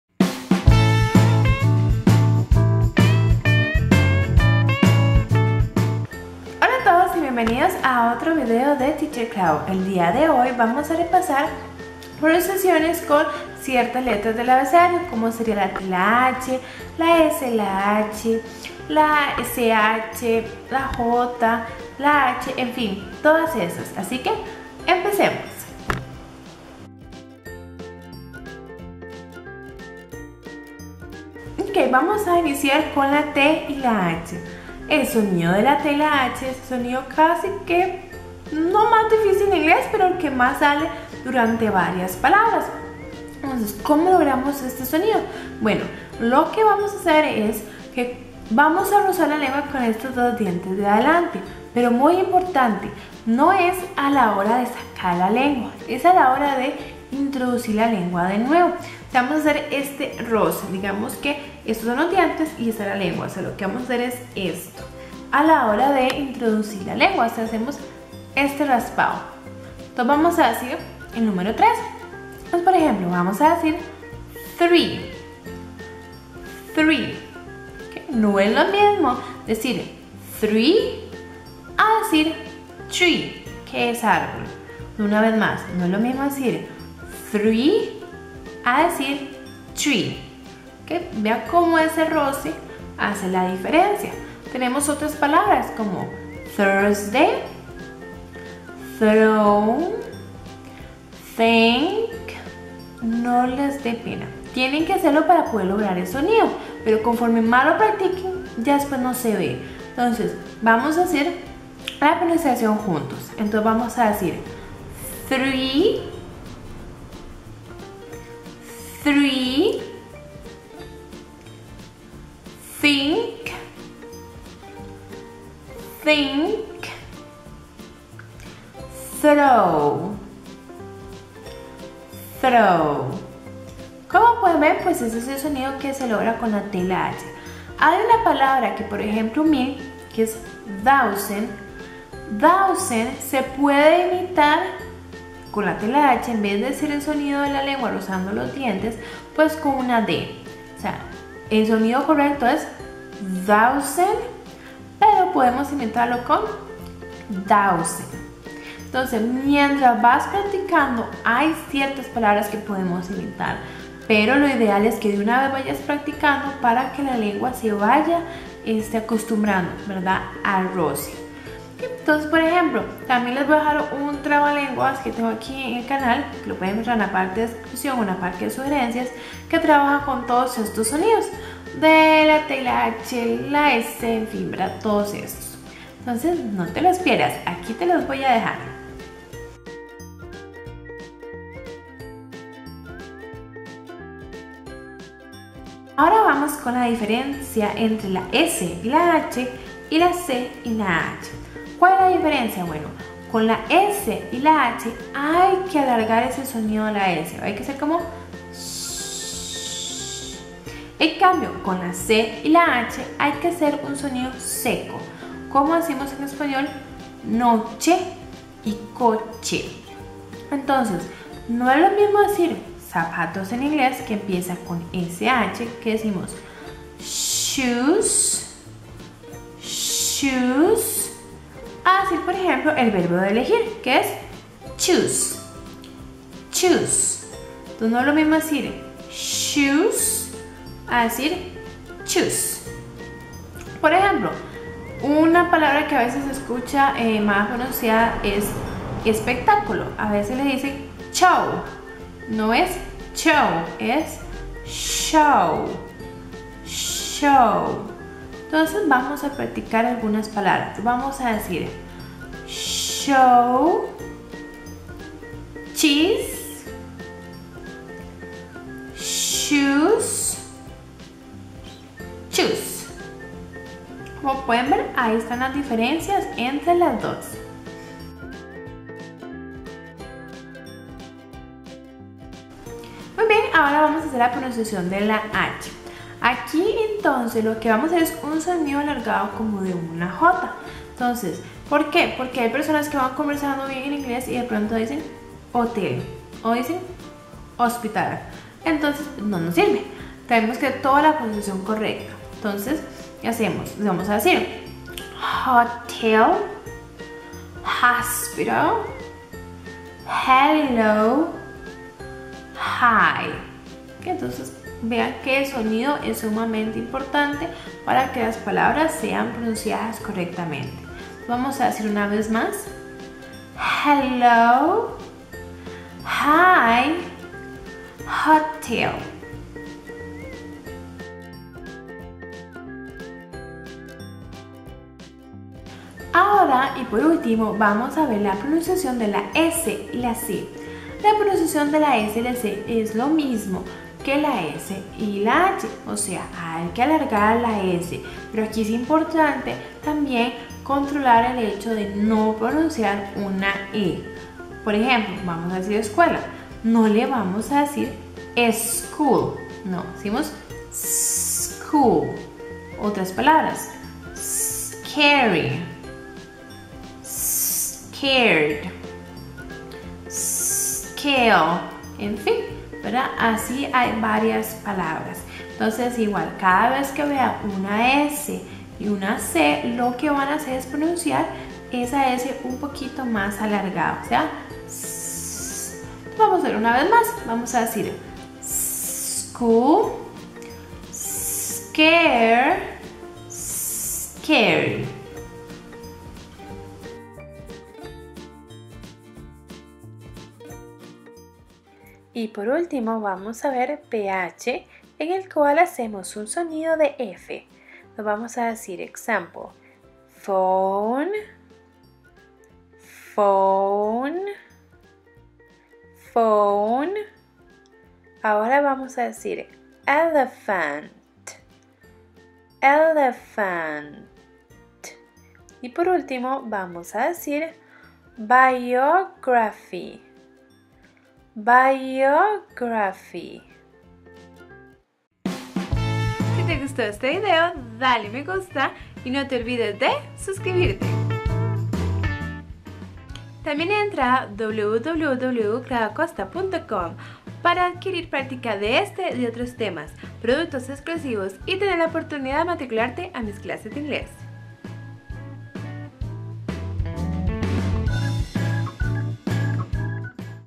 Aprende en Inglés Los Sonidos: "Th", "Sh", "Ch", "H" ,"Sc", "Ph".
El dia de hoy te traigo los sonidos más comunes en inglés.

Frases y Proverbios en Ingles

Halloween Tongue Twisters!